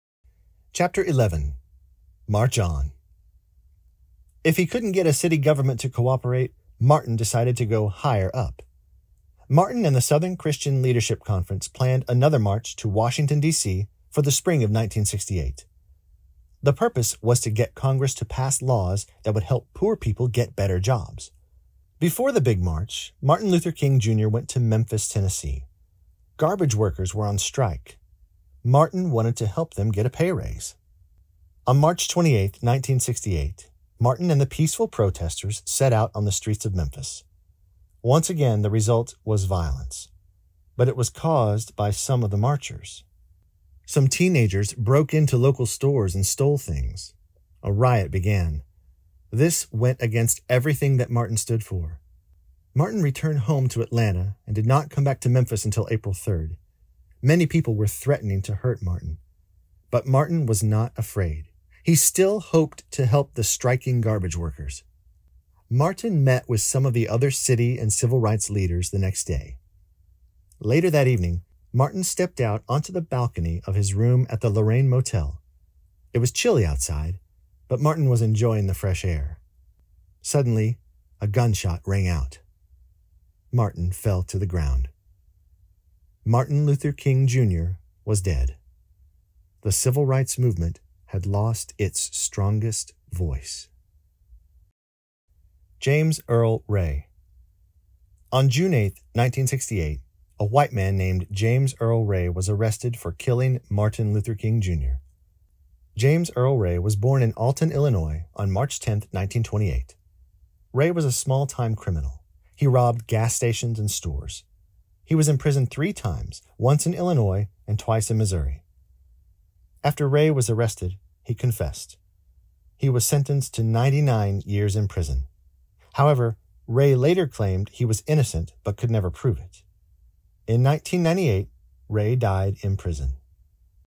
Who Was Martin Luther King, Jr? - Chapter 11A / James Earl Ray
*Yes, there are two "Chapter 11s" in this book!*

By Bonnie Bader
Grosset & Dunlap